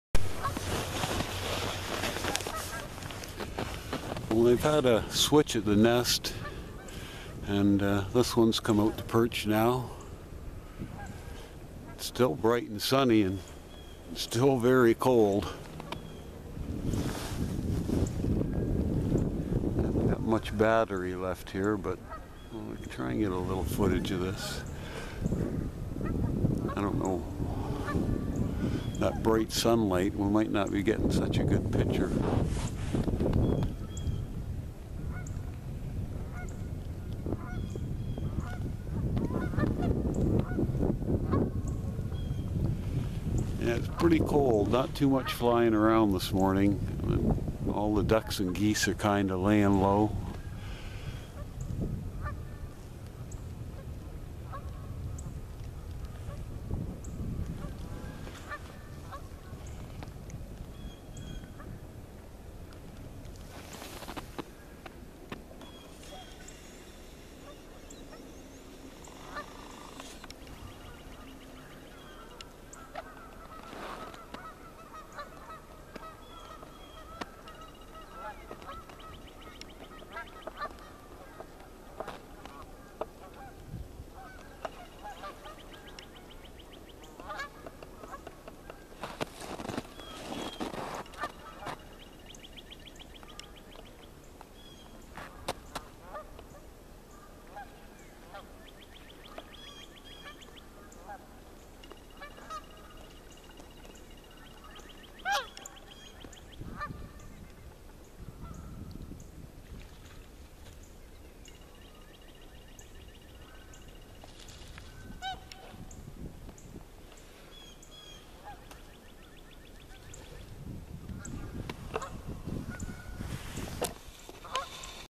Grand River Eagles
Friday morning checking on the local Bald Eagles